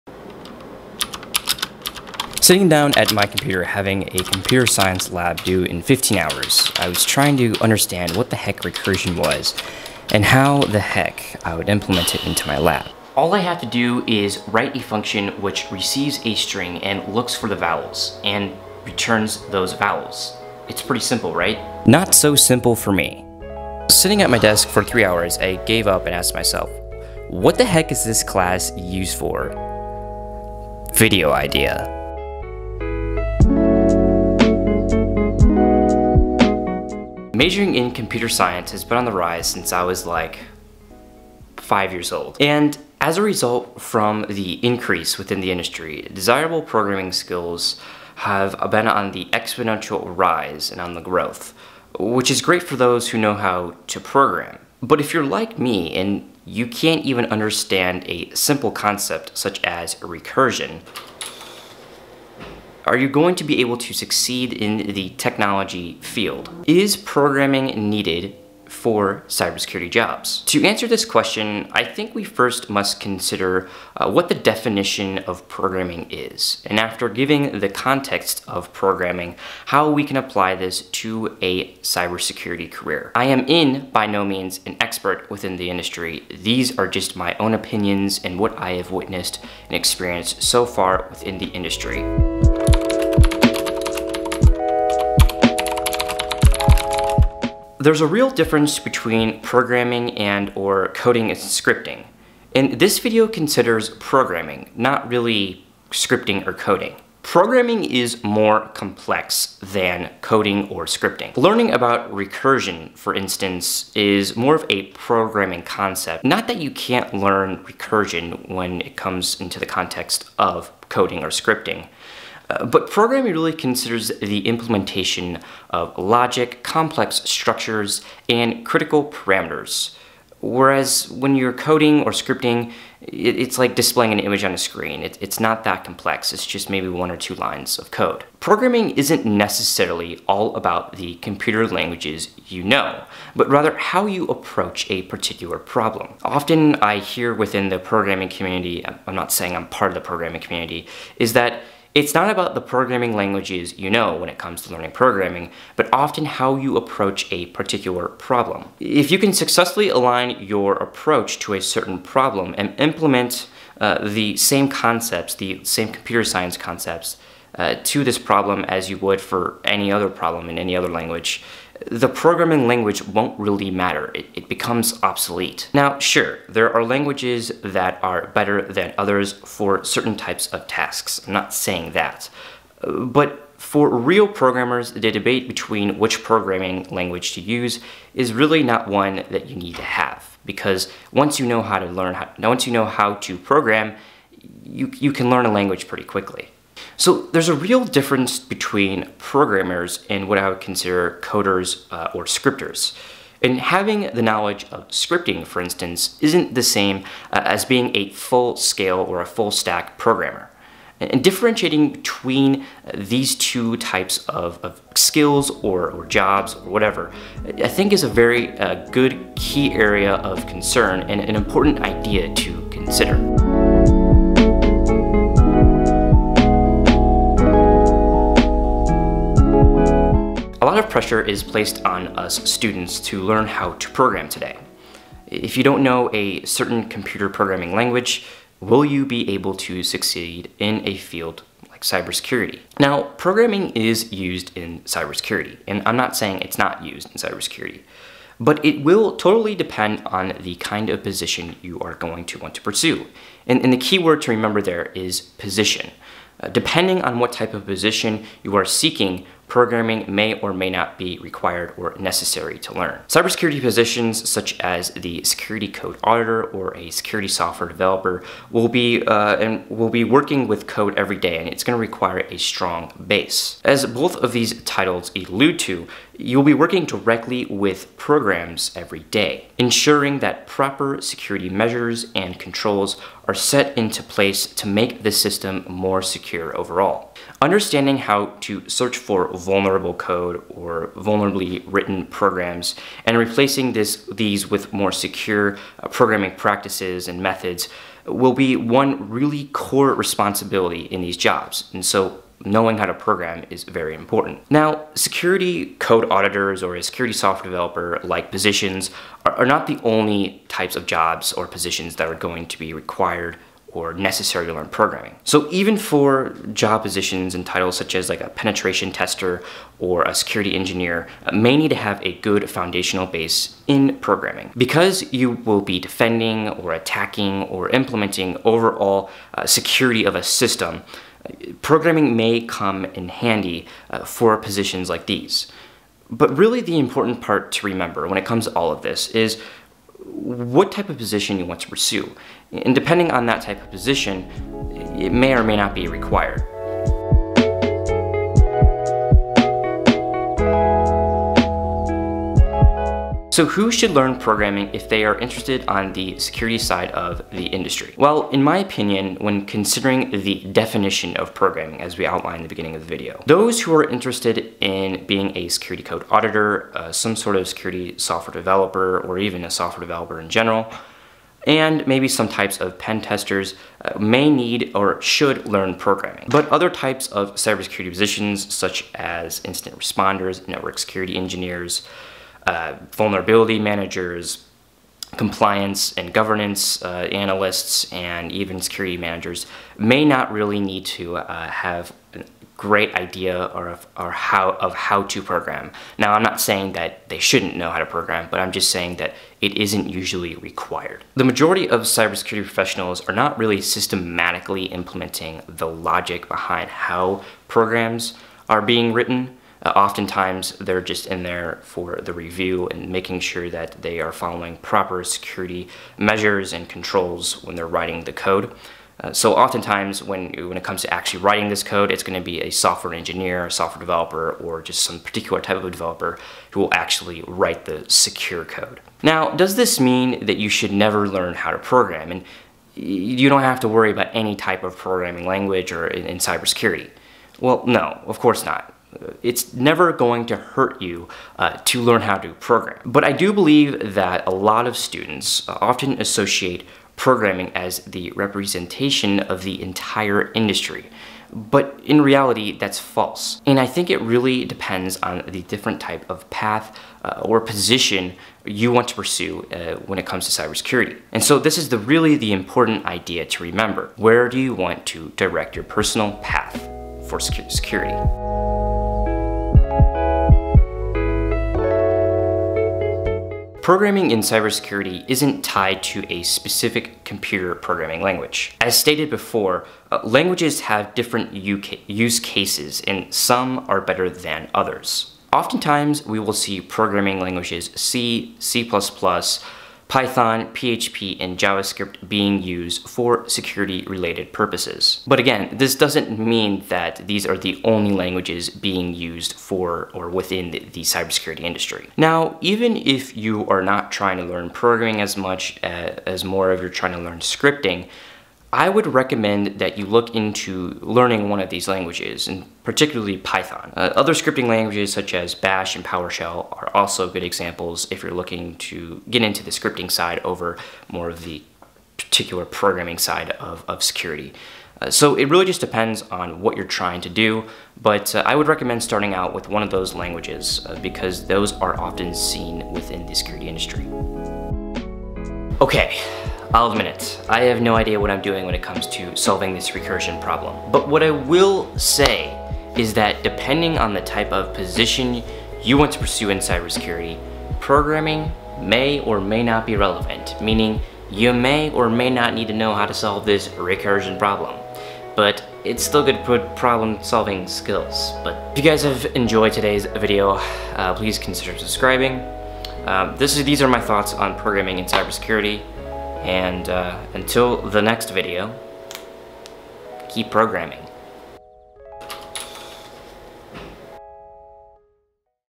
Programming in Cybersecurity
I was struggling with a Computer Science Lab this past week, we were covering recursion and I couldn't seem to grasp the concept...  After sitting for 3+ hours at my desk, I asked myself an important question, "where is programming used in cybersecurity?" Although I have done videos in the regarding programming, I take a deeper look into defining what "programming" is, where it's used in cybersecurity, whether you should learn it for your type of position, and common programming languages used in the cybersecurity industry.

Join the Email List for More Information and Exclusive Updates!

Gear:
Laptop (Lenovo X1 Carbon Ultrabook)
Standing Desk (FlexiSpot M3B 47")